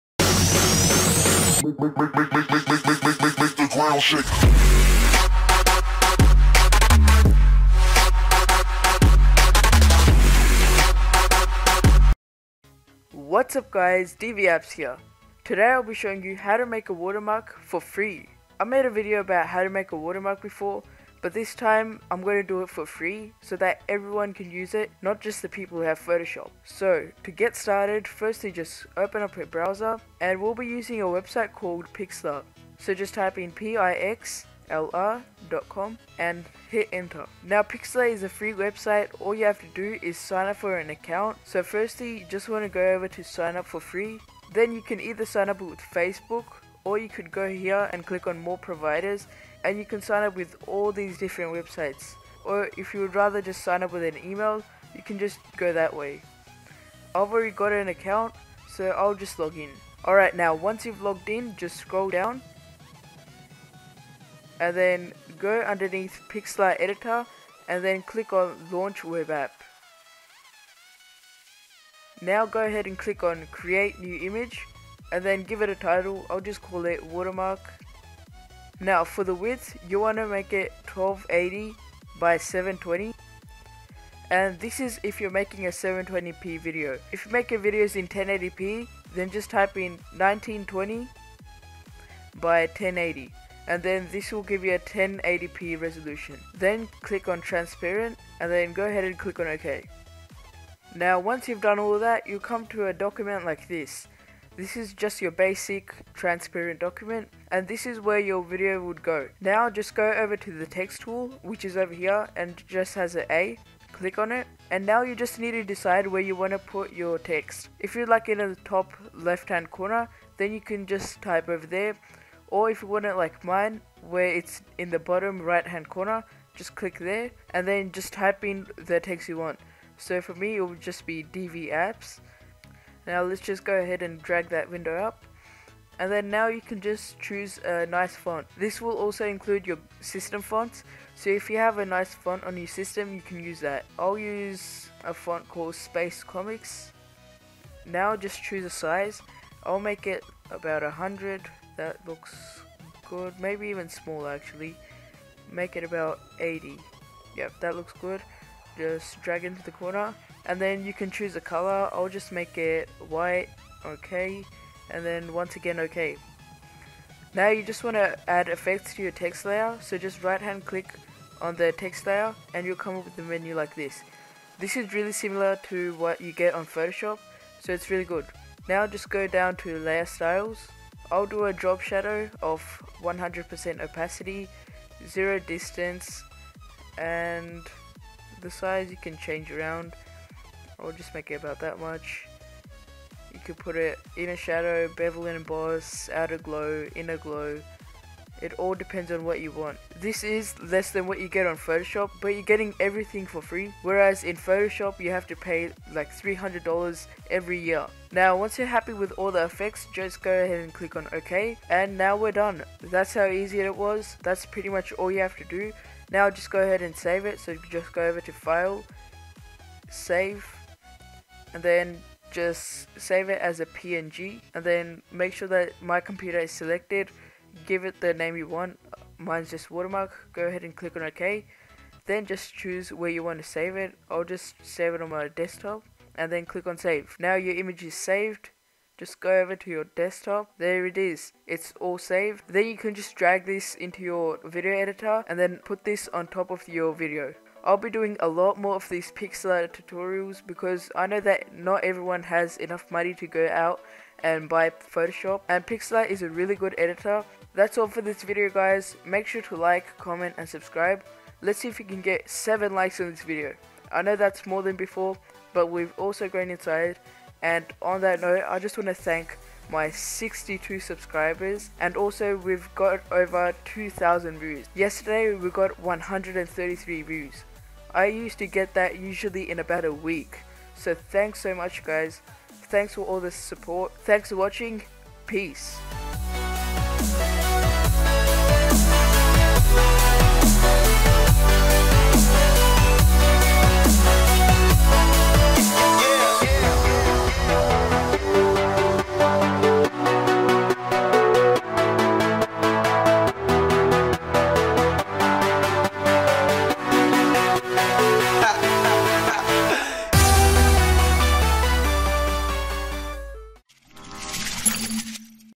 How To Make A Watermark For Your Videos For FREE! Pixlr Tutorial
What’s up guys, DvApps here. Today I'll be showing you How To Make A Watermark For Your Videos For FREE using a free web app called Pixlr. Pixlr is a great alternative to Adobe Photoshop and has many of the same functions, but"toned down". It's great for beginners and professionals, who just don't have enough money to go and buy Adobe Photoshop CS5/CS6/CC.

A special thanks to my 62 subscribers, it may not seem a lot but for me it is so, thanks.

Also...